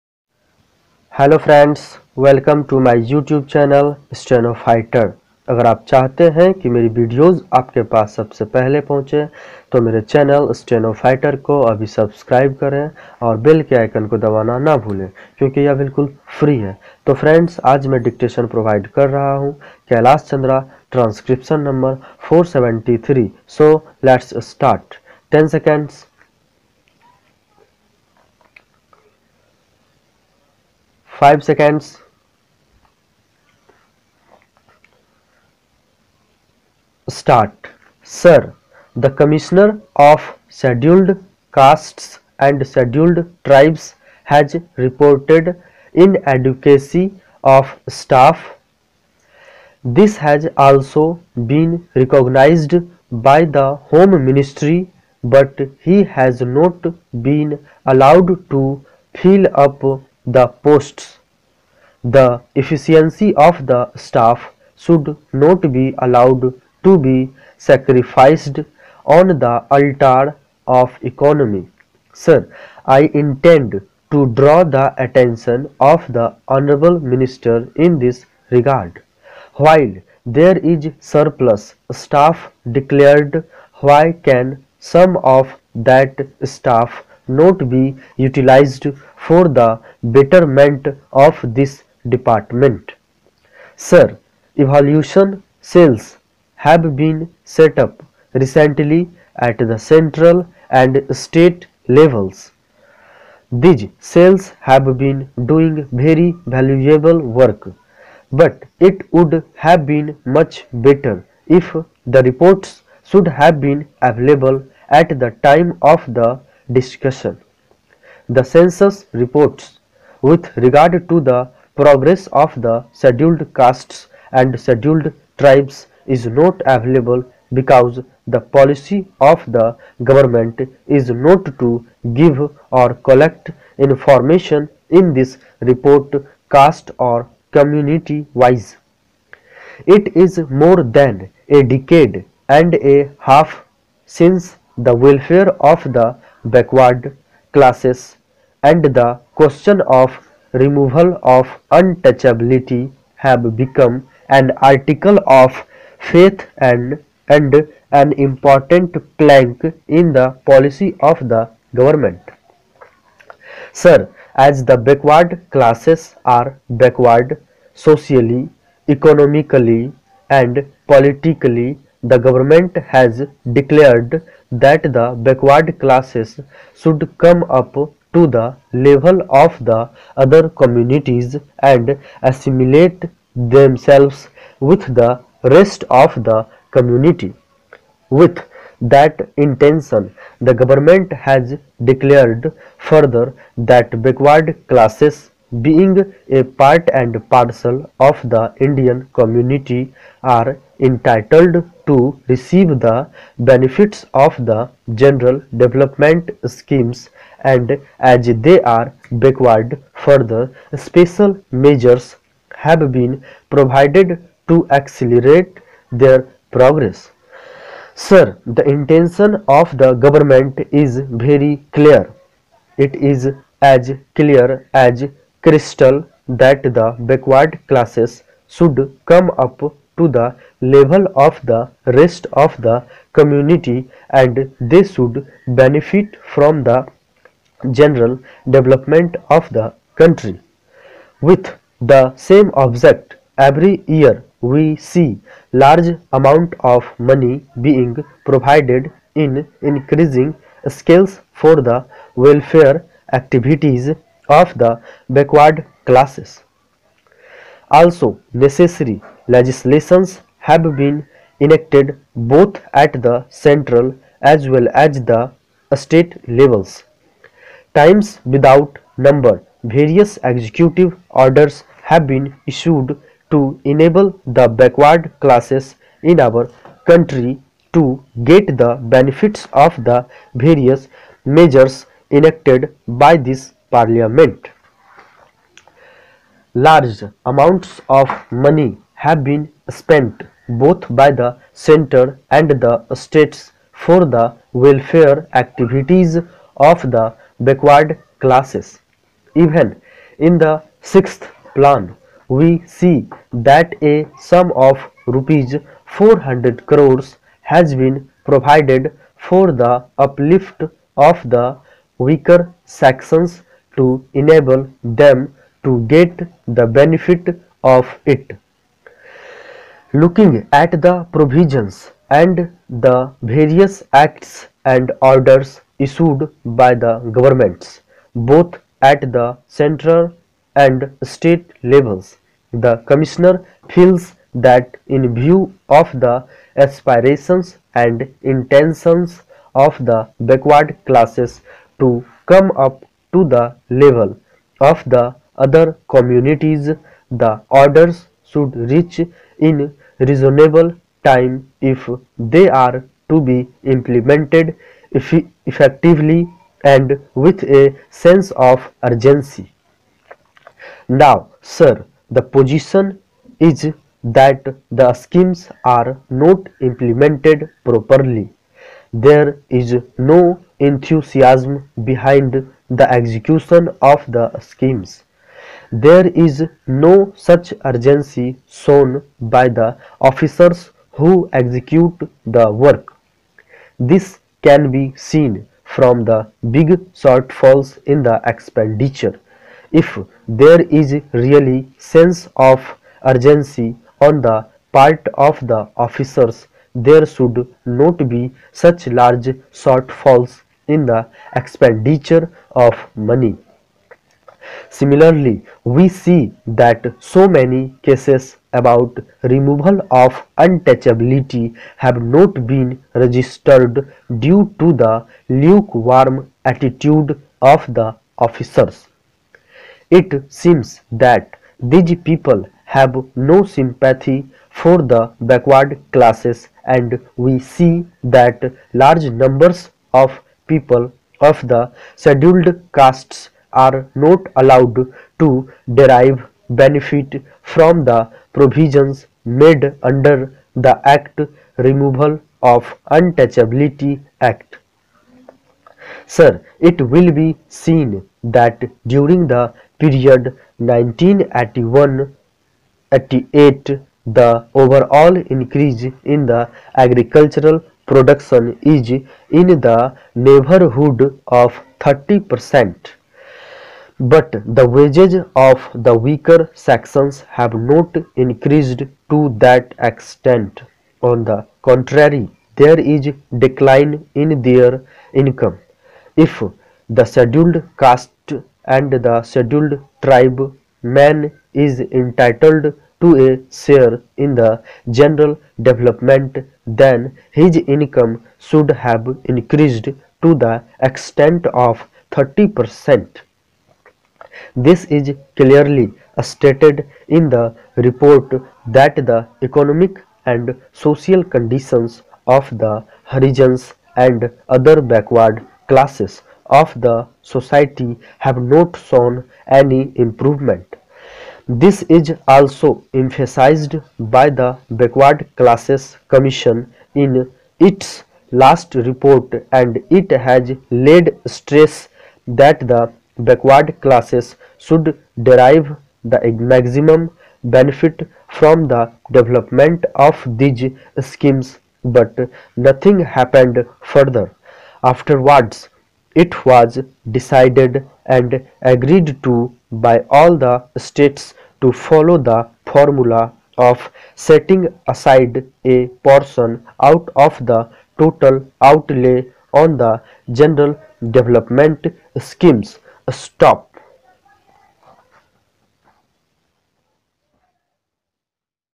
Kailash Chandra dictation | Transcription No. 473 | @ 95WPM | 840 words
This video has kailash chandra shorthand dictation @95wpm. This video useful in SSC upcoming skill test, ASRB skill test, DSSSB skill test and also useful in other shorthand exam. Thank u so much watching my video.